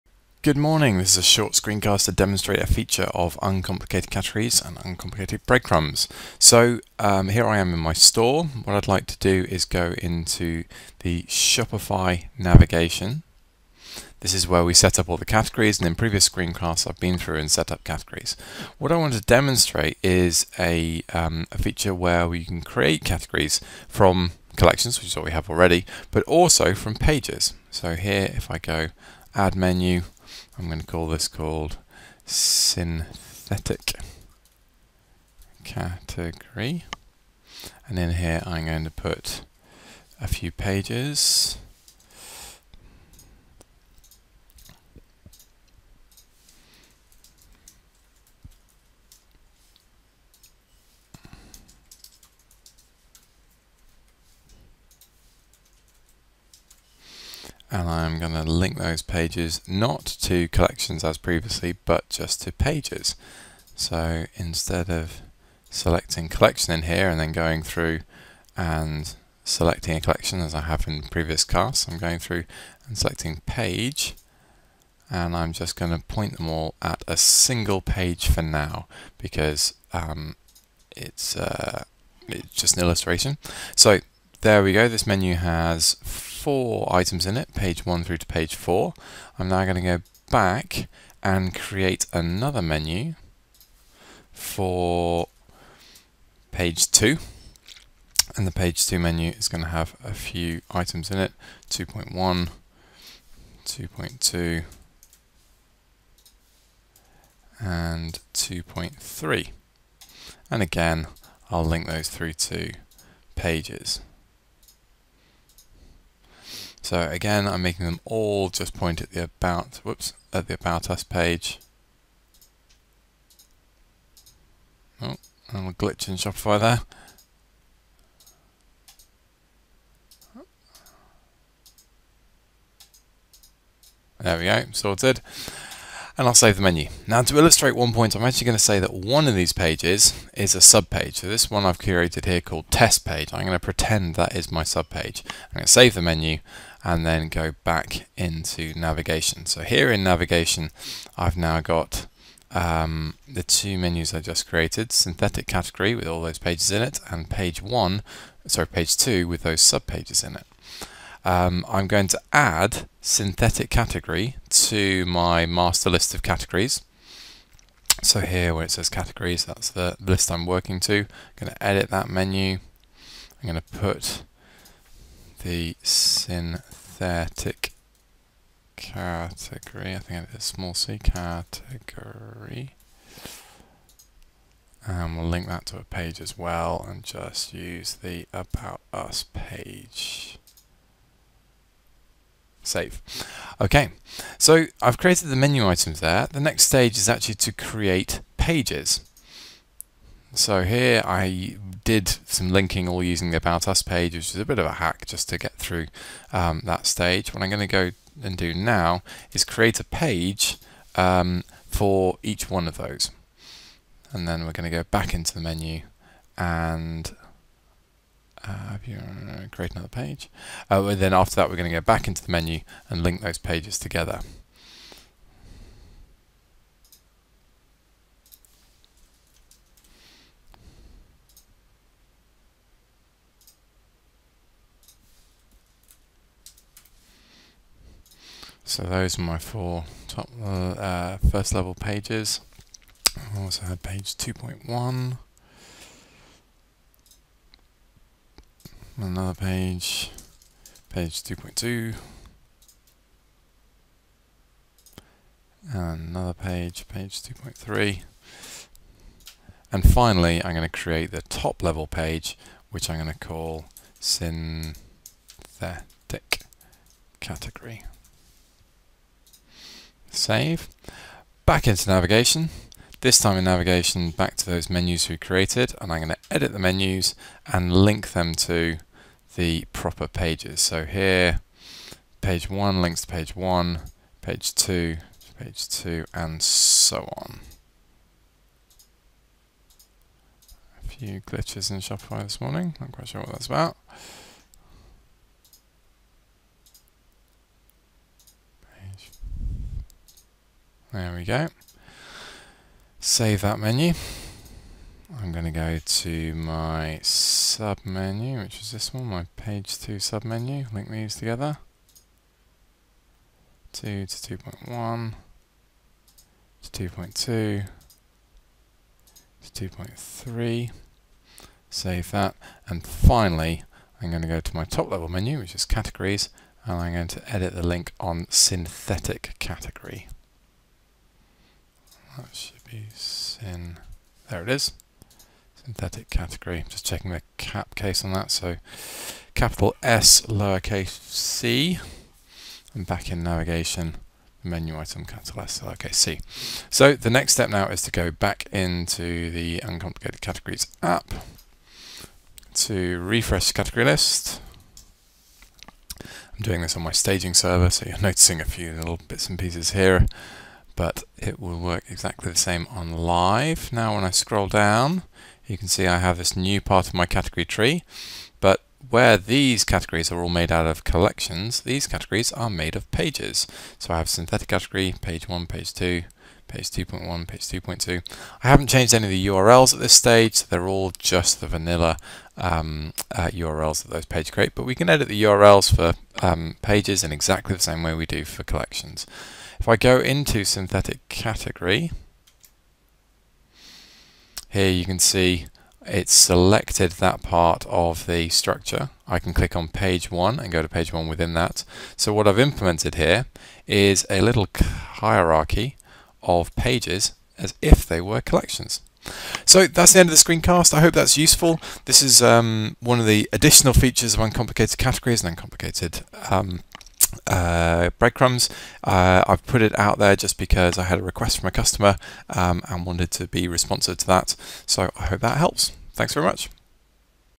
Creating categories from Shopify pages
This screencast shows how to link together pages to form a navigation tree.